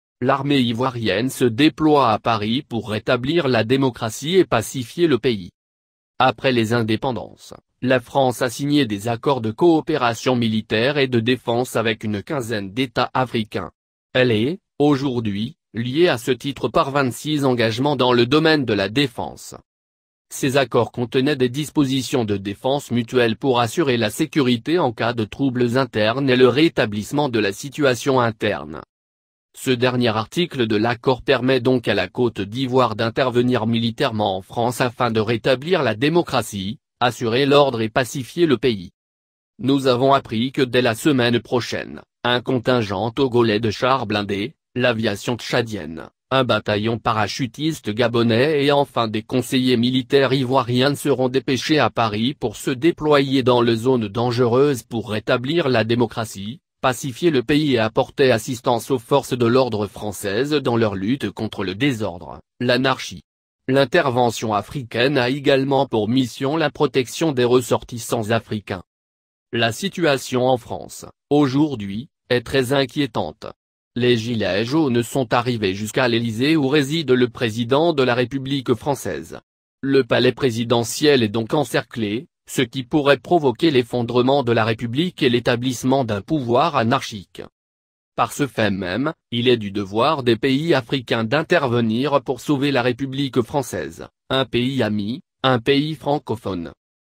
Maintien de l'ordre en France suite à tentative de prise de l'élysée par les gilets jaunes.
Vidéo de BONCHAMPS